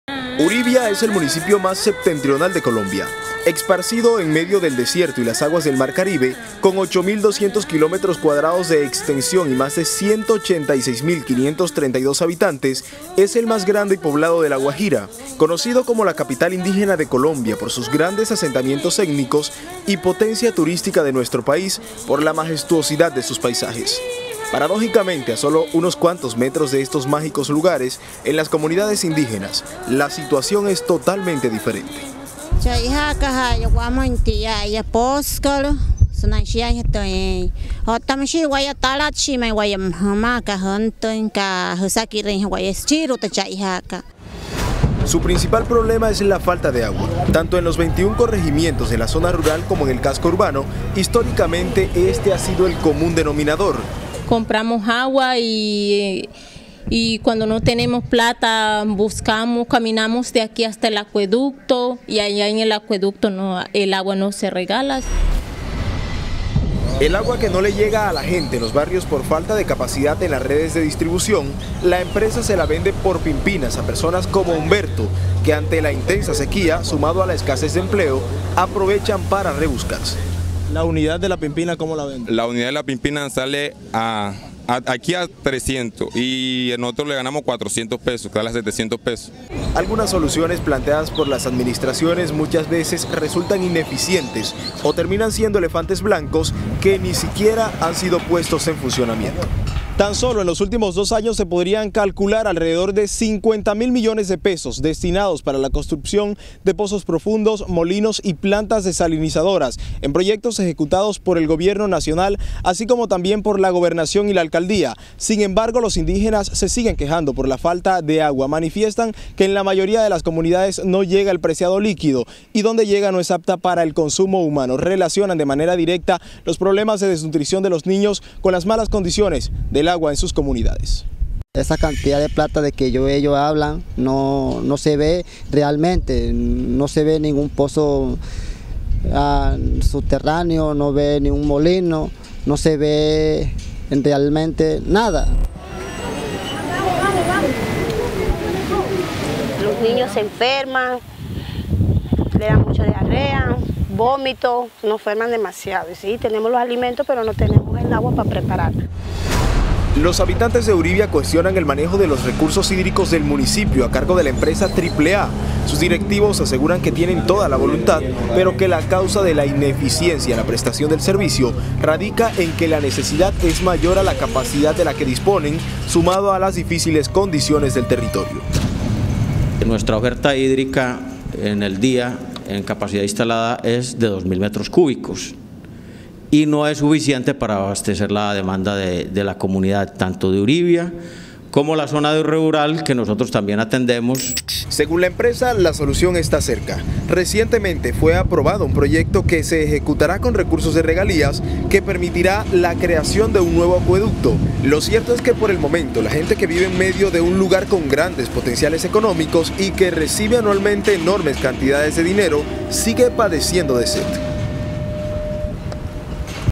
Uribia, un paraíso con sed en La Guajira
Uribia es el municipio más septentrional de Colombia, esparcida en medio del desierto y las aguas del mar Caribe con 8.200 kilómetros cuadrados de extensión y más de 186.532 habitantes, es el más grande poblado de La Guajira.